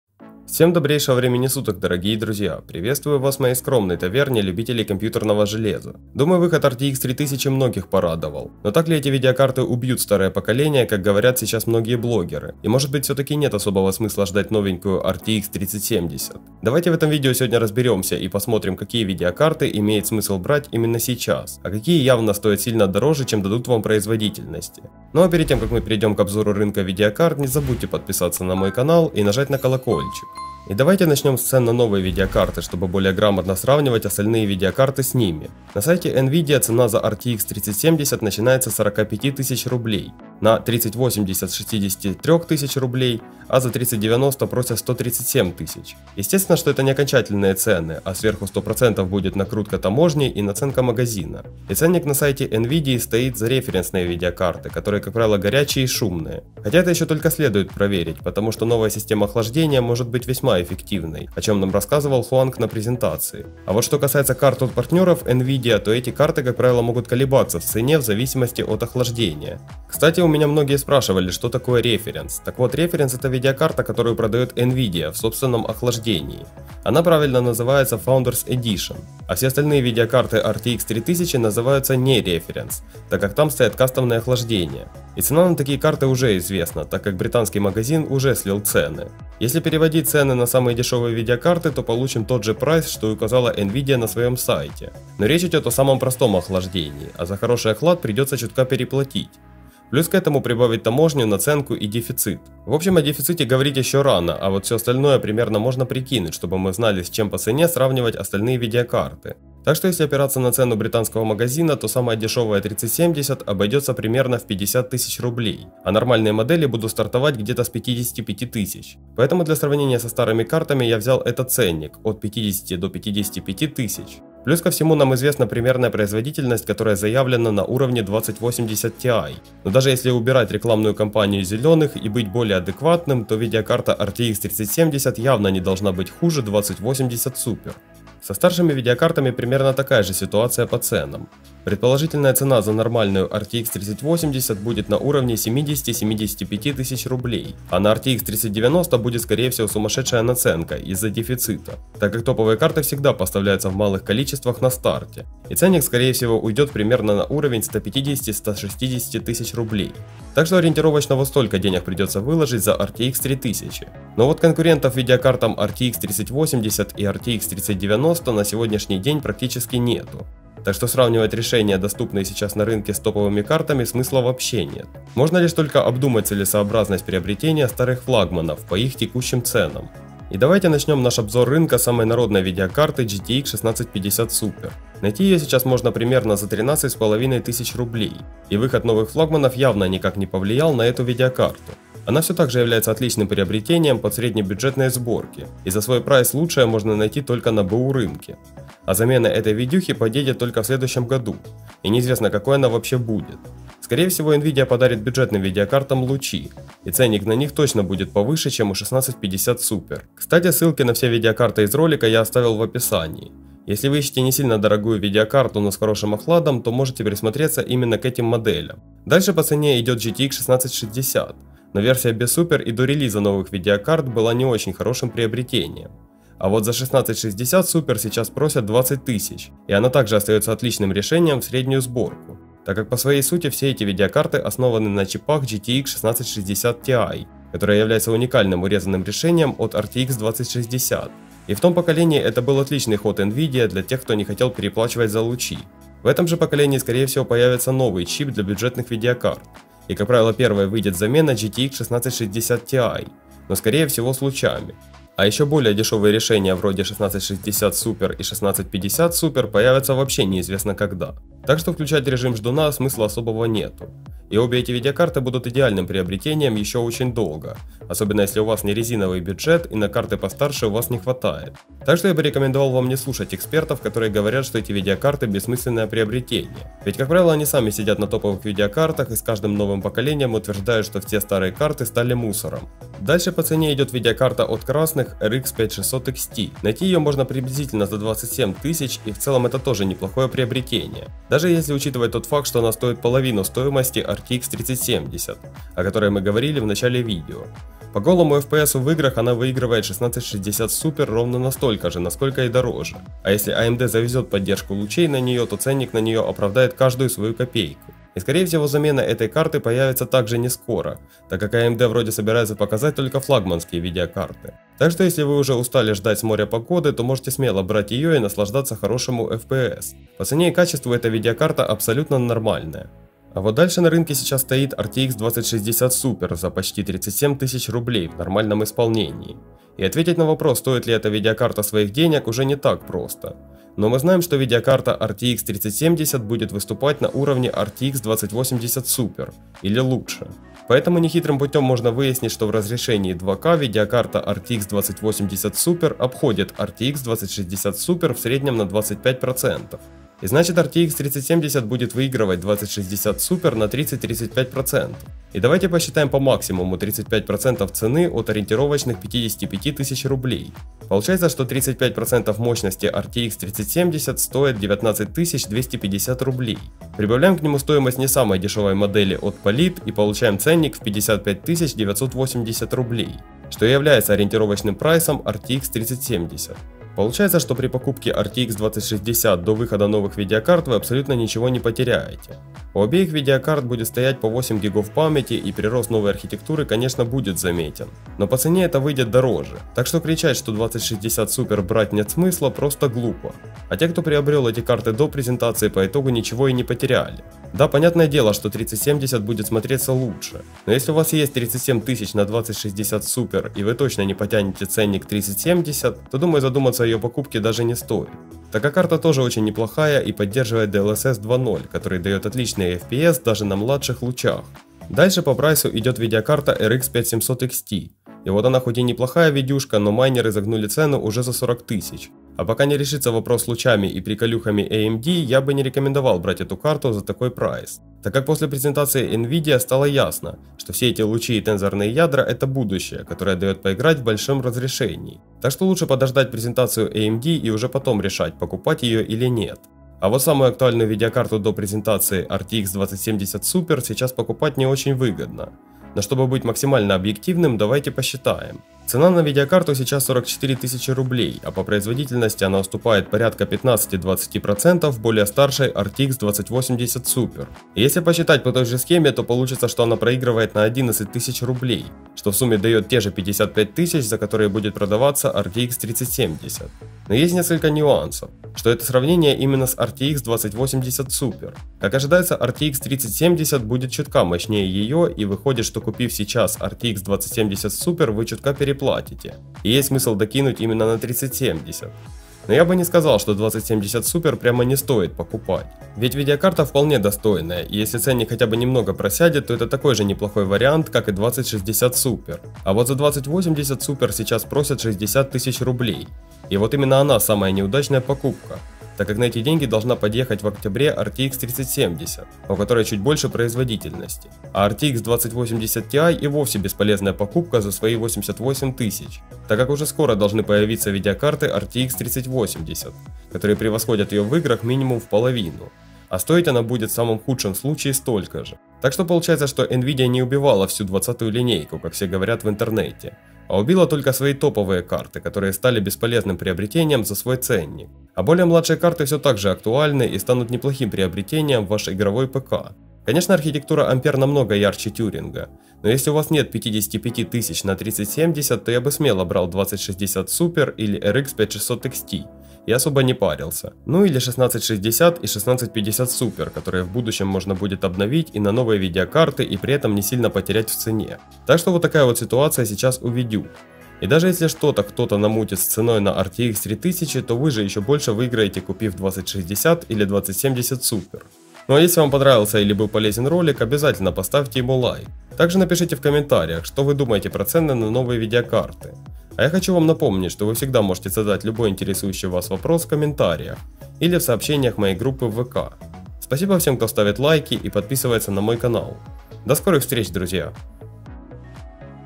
RTX 3000 - выбор видеокарты до выхода RTX 3070
Стоит ли покупать старые видеокарты до выхода RTX 3000 и Big Navi? Разбираемся в рынке видеокарт и смотрим на цены видеокарт до появления в продаже RTX 3070. Решим какую видеокарту лучше купить в 2020 году, кроме лучших видеокарт на Nvidia Ampere. По производительности, RTX 3090 и RTX 3080 на рынке видеокарт нет конкурентов. Поэтому рассмотрим только целесообразность ждать их по цене. А вот цены на старые Nvidia RTX сейчас не маленькие, поэтому я решил выбрать самые адекватные видеокарты до выхода RTX 3070 и ответить на вопрос есть ли смысл брать видеокарту до появления RTX 3000.

0:00-0:30 Приветствие
0:30-1:10 Цена на RTX 3090, RTX 3080, RTX 3070
1:10-1:25 Референсы RTX 3000
1:25-3:00 Цены на не рефы RTX 3070 и 3080
3:00-5:23 Бюджетные видеокарты против RTX 3000
5:23-8:12 Среднячки против RTX 3070
8:12-8:39 Про топовые Nvidia RTX
8:39-9:21 Выводы о покупке Nvidia Ampere
9:21-9:41 Концовка